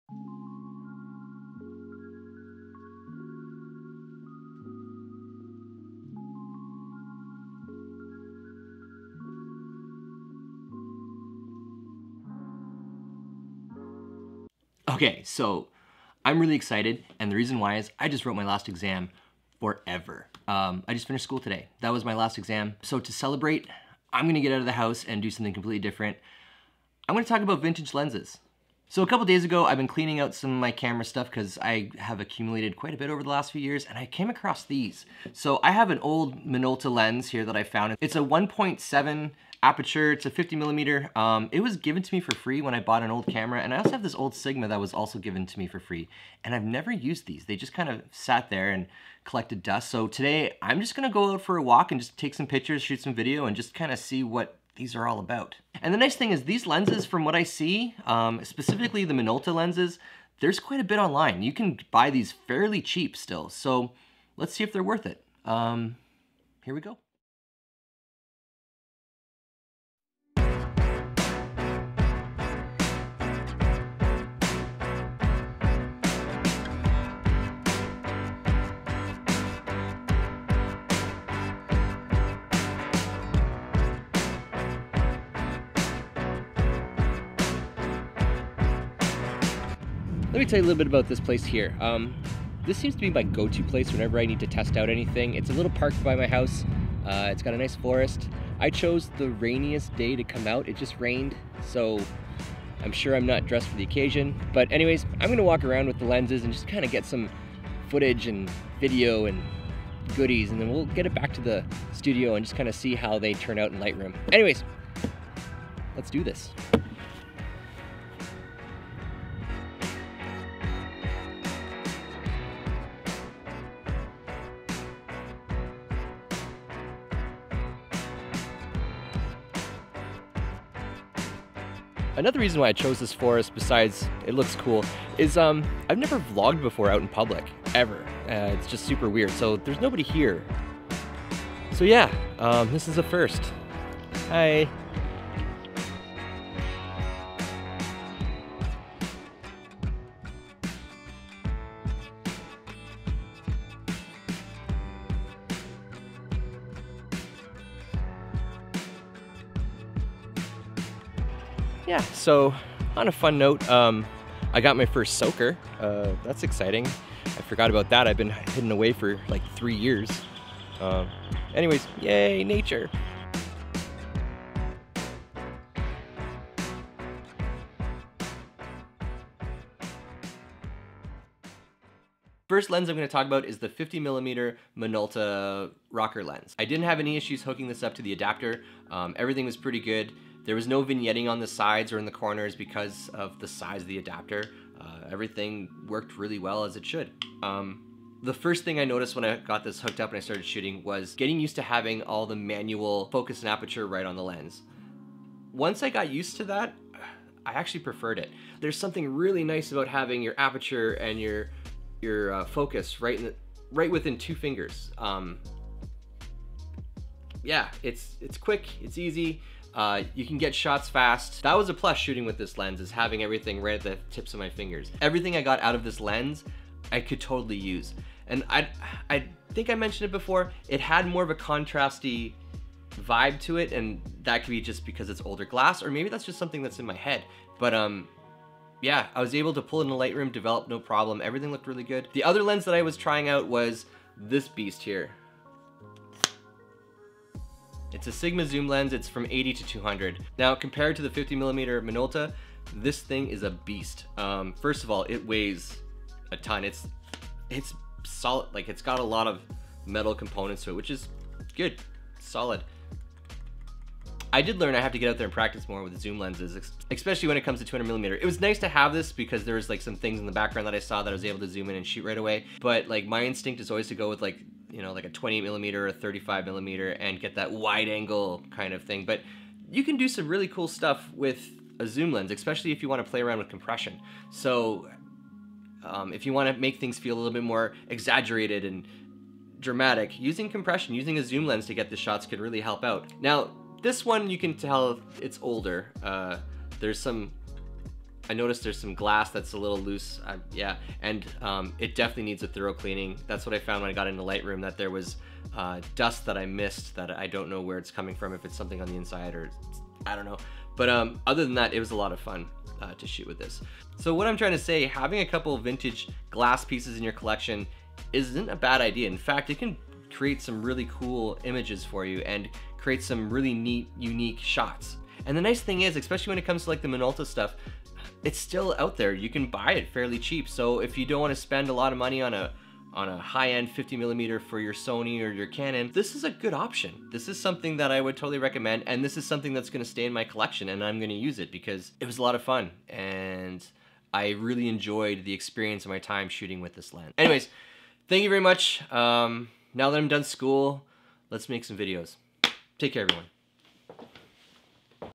Are vintage lenses still worth it
I decided to dust off my old vintage lenses I got years ago. The first vintage lens that I played around with is a Minolta Rokkor 50mm from 1973. I love how using vintage lenses can add a unique perspective on photography.